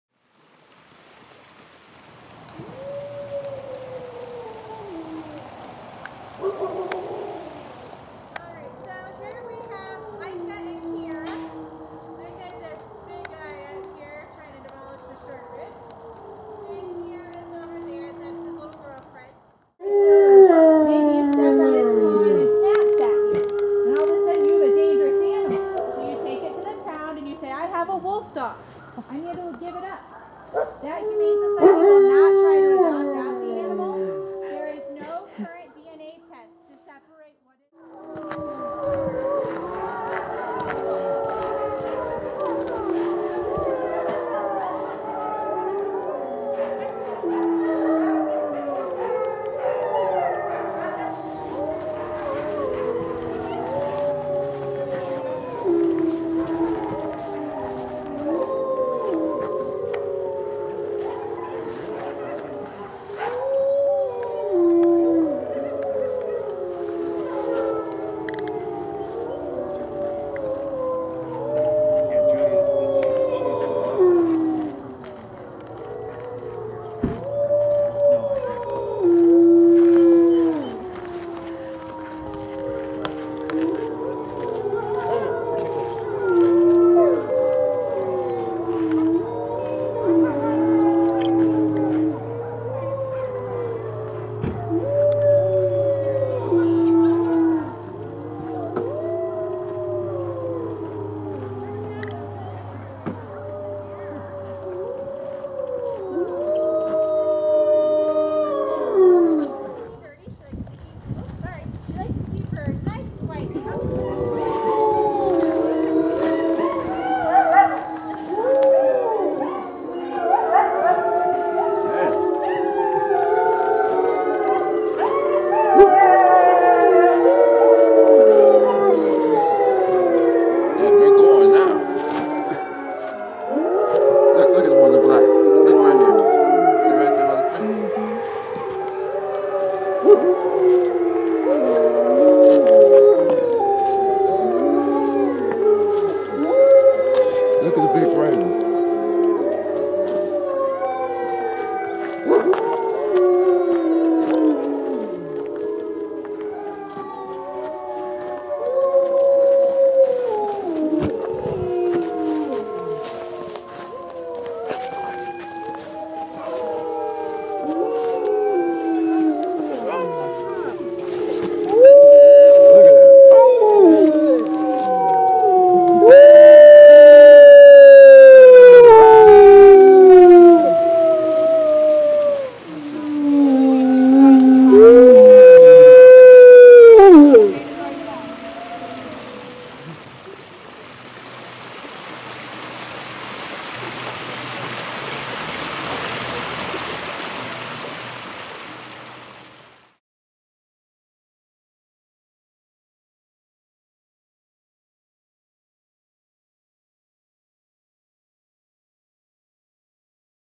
Wolf Howls! (Full Moon)
November 17th 2013--Full Moon in Taurus
Colorado Wolf & Wildlife Center

FantomCat Photography